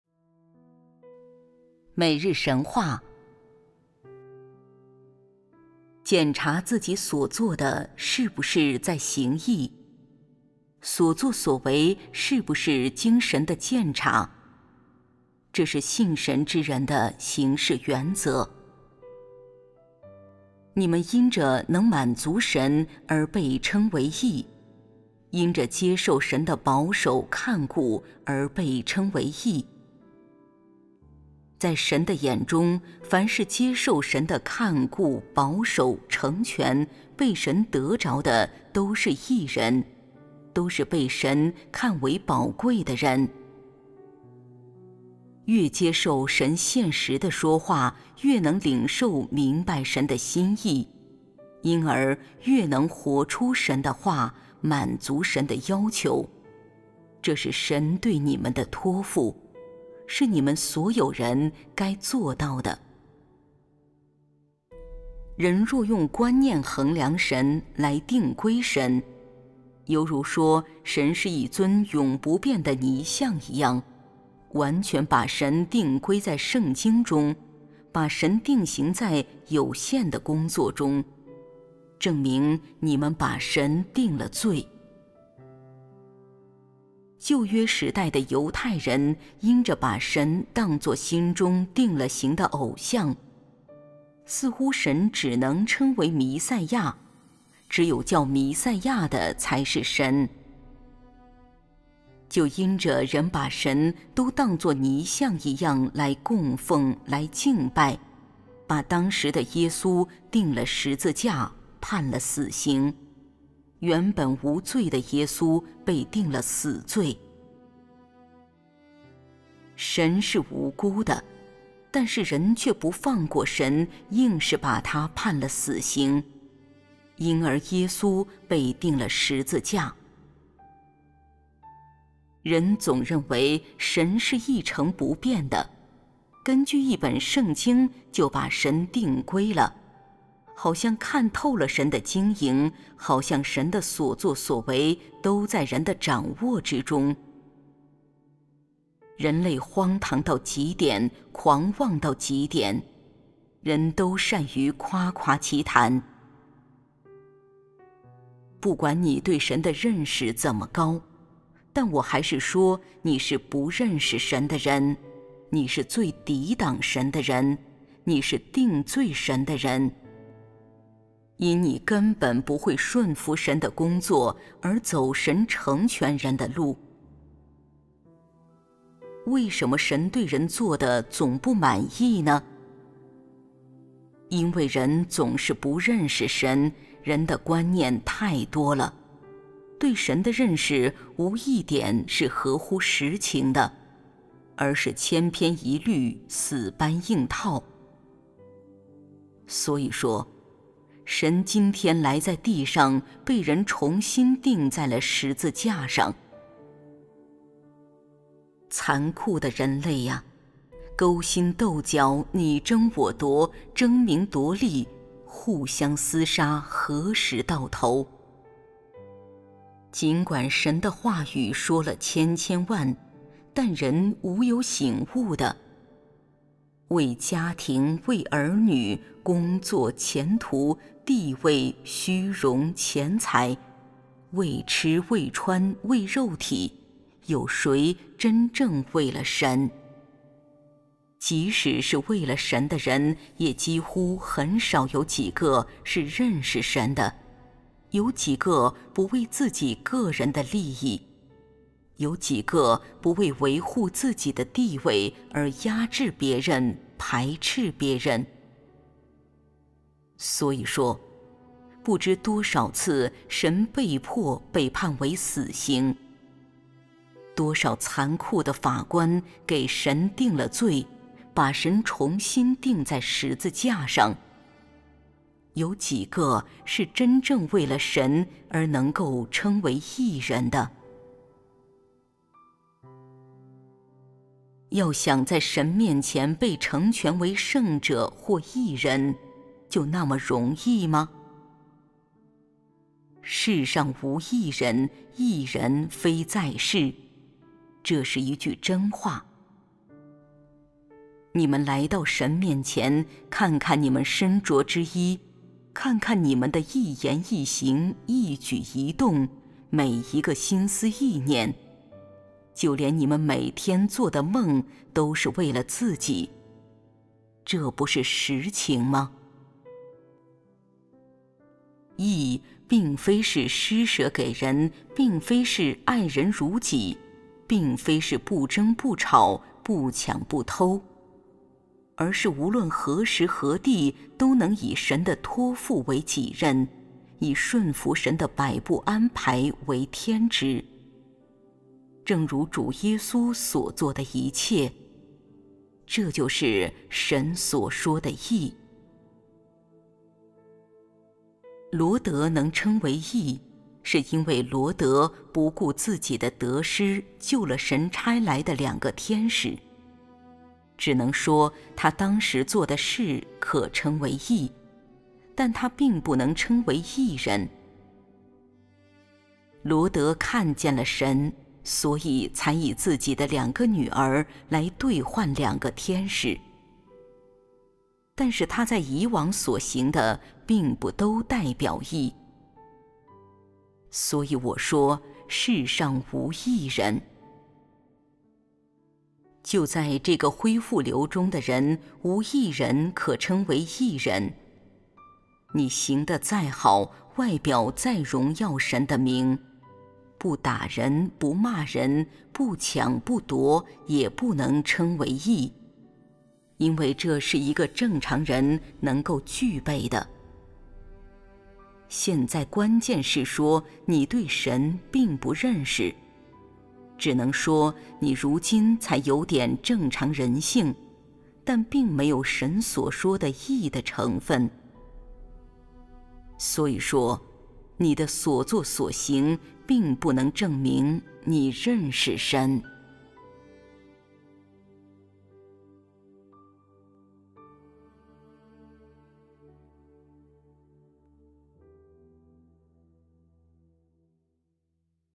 每日神話 - 揭示人類敗壞系列 選段328
你聽到主再來的發聲説話了嗎？歡迎進入我們官方網站書籍頁面，聆聽神的聲音，跟上羔羊的脚踪。

觀看更多每日神話視頻：
每日神話 - 揭示人類敗壞系列

　　檢查自己所做的是不是在行義，所作所為是不是經神的鑒察，這是信神之人的行事原則。你們因着能滿足神而被稱為義，因着接受神的保守、看顧而被稱為義。在神的眼中，凡是接受神的看顧、保守、成全被神得着的都是義人，都是被神看為寶貴的人。越接受神現實的説話，越能領受明白神的心意，因而越能活出神的話，滿足神的要求，這是神對你們的托付，是你們所有人該做到的。人若用觀念衡量神，來定規神，猶如説神是一尊永不變的泥像一樣，完全把神定規在聖經中，把神定形在有限的工作中，證明你們把神定了罪。舊約時代的猶太人因着把神當作心中定了型的偶像，似乎神只能稱為彌賽亞，只有叫彌賽亞的才是神，就因着人把神都當作泥像（無生命）一樣來供奉、來敬拜，把當時的耶穌釘了十字架，判了死刑，原本無罪的耶穌被定了死罪。神是無辜的，但是人却不放過神，硬是把他判了死刑，因而耶穌被釘了十字架。人總認為神是一成不變的，根據一本聖經就把神定規了，好像看透了神的經營，好像神的所作所為都在人的掌握之中。人類荒唐到極點，狂妄到極點，人都善于誇誇其談。不管你對神的認識怎麽高，但我還是説你是不認識神的人，你是最抵擋神的人，你是定罪神的人，因你根本不會順服神的工作而走神成全人的路。為什麽神對人做的總不滿意呢？因為人總是不認識神，人的觀念太多了，對神的認識無一點是合乎實情的，而是千篇一律，死搬硬套。所以説，神今天來在地上，被人重新釘在了十字架上。殘酷的人類啊！勾心鬥角、你争我奪、争名奪利、互相厮殺何時到頭？儘管神的話語説了千千萬，但人無有醒悟的，為家庭，為兒女、工作、前途、地位、虚榮、錢財，為吃為穿為肉體，有誰真正為了神？即使是為了神的人也幾乎很少有幾個是認識神的，有幾個不為自己個人的利益？有幾個不為維護自己的地位而壓制别人、排斥别人？所以説，不知多少次神被迫被判為死刑，多少殘酷的法官給神定了罪，把神重新釘在十字架上，有幾個是真正為了神而能够稱為義人的？
　　要想在神面前被成全為聖者或義人就那麽容易嗎？「世上無義人，義人非在世」，這是一句真話。你們來到神面前，看看你們身着之衣，看看你們的一言一行、一舉一動、每一個心思意念，就連你們每天做的夢都是為了自己，這不是實情嗎？「義」并非是施捨給人，并非是愛人如己，并非是不争不吵、不搶不偷，而是無論何時何地都能以神的托付為己任，以順服神的擺布安排為天職，正如主耶穌所作的一切，這就是神所説的義。羅得能稱為義是因為羅得不顧自己的得失救了神差來的兩個天使，只能説他當時做的事可稱為義，但他并不能稱為義人。羅得看見了神，所以才以自己的兩個女兒來兑换兩個天使，但是他在以往所行的并不都代表義，所以我説「世上無義人」。就在這個恢復流中的人無一人可稱為義人，你行得再好，外表再榮耀神的名，不打人不駡人，不搶不奪，也不能稱為義，因為這是一個正常人能够具備的。現在關鍵是説你對神并不認識，只能説你如今才有點正常人性，但并没有神所説的義的成分，所以説你的所做所行并不能證明你認識神。
                                                                                                                  ——摘自《話在肉身顯現》